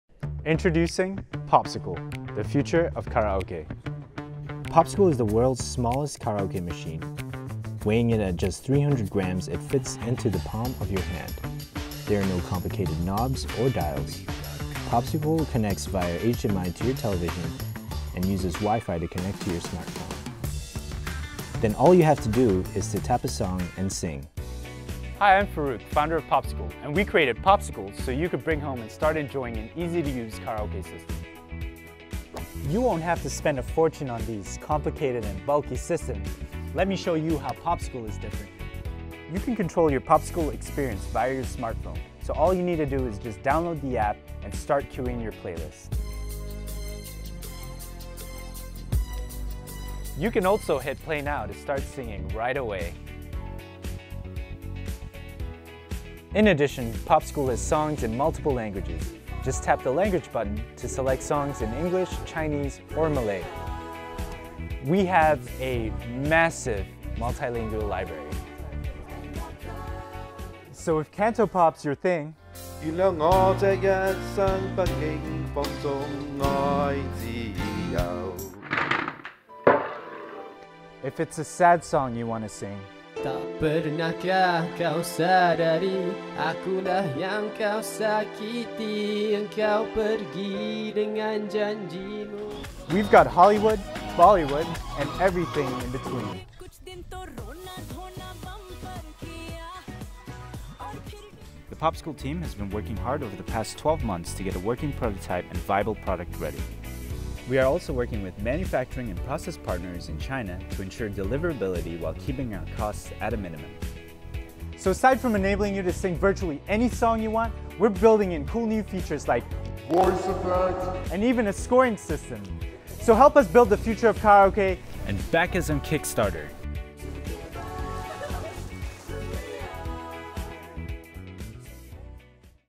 Popsical - the smartest and smallest streaming karaoke system
Popsical is building the future of Karaoke with thoughtfully designed hardware and software. Plug into any sound system and TV for a fluid Karaoke-on-demand experience.

It is lightweight, stylish, and fits right into any environment. The Popsical App allows users to queue the playlist right from their smartphones.

All you need is an internet connection to browse through an ever-growing music library of songs in multiple languages. The library stays constantly updated so you can sing to the latest hits.